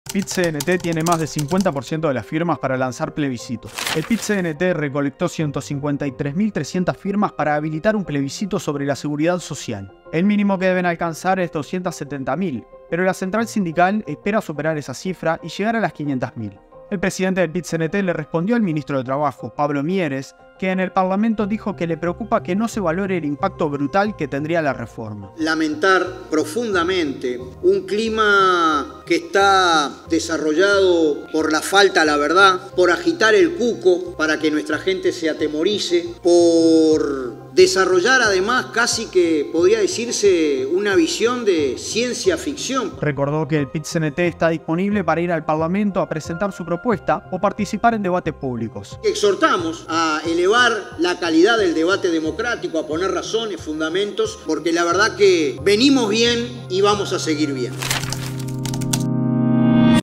PIT-CNT alcanzó más de la mitad de las firmas necesarias para el plebiscito de seguridad social
La central sindical uruguaya logró 153.000 firmas de las 270.000 que debe obtener para poder habilitar el plebiscito sobre una nueva reforma de la seguridad social. El presidente del PIT-CNT, Marcelo Abdala, criticó que el gobierno utilice “cucos” para atemorizar a la población